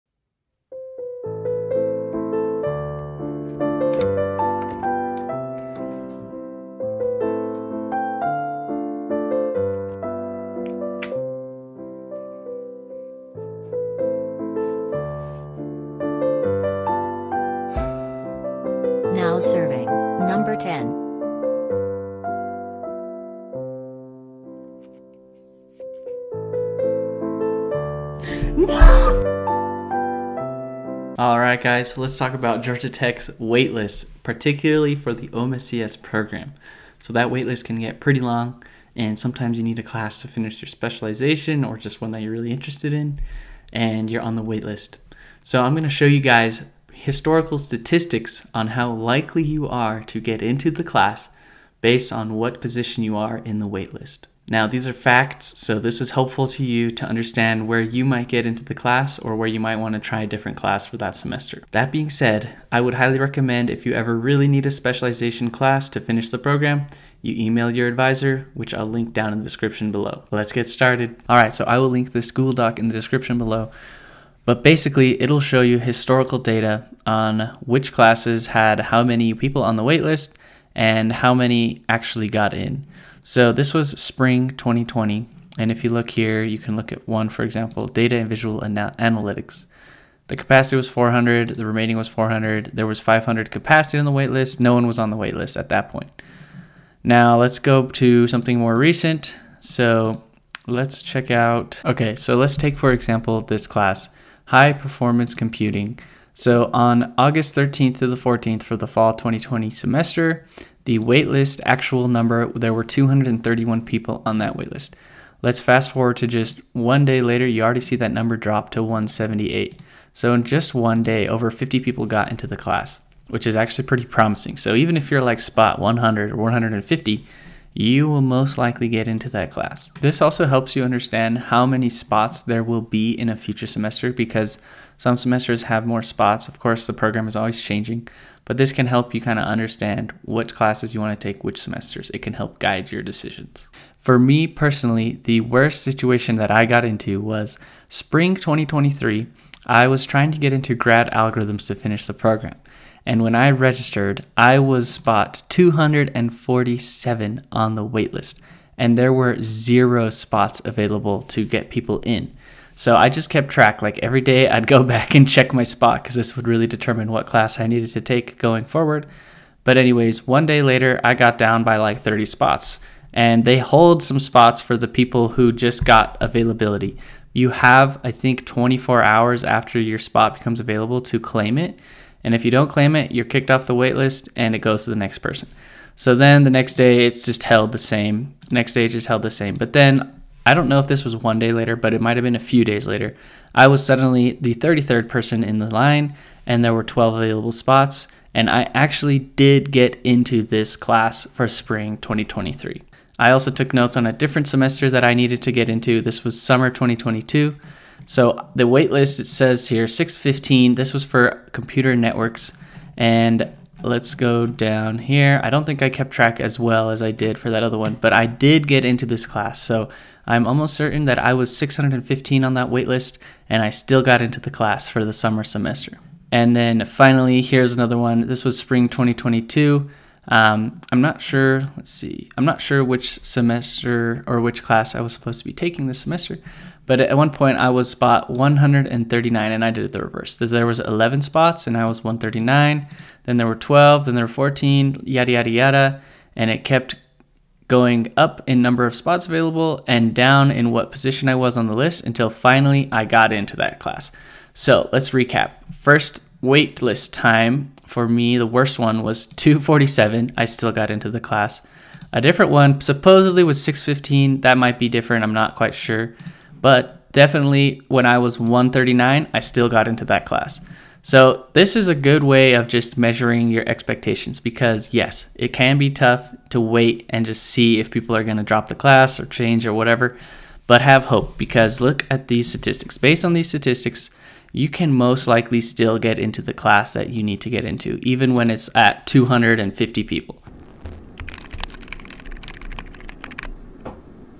Georgia Tech OMSCS Waitlist: Will you get in? (Historical Data Shown!!)
00:00 Intro
01:11 Historical Data
03:22 My own data
04:50 Outro